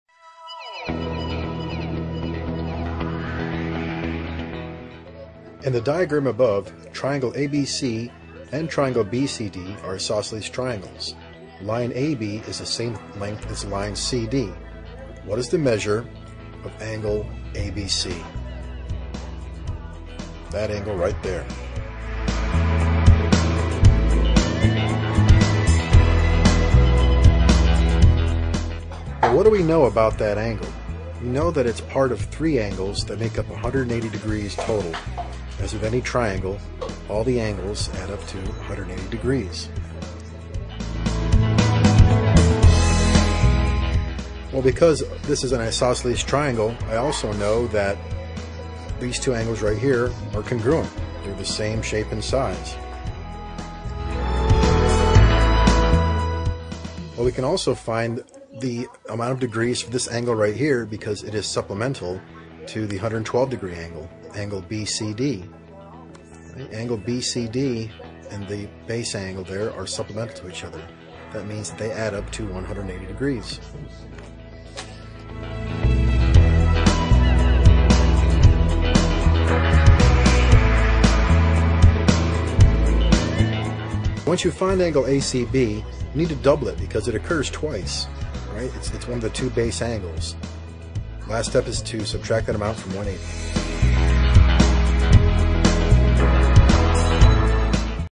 Finding an Angle in an Isoceles Triangle
Find the angle amount indirectly when given two isosceles triangles.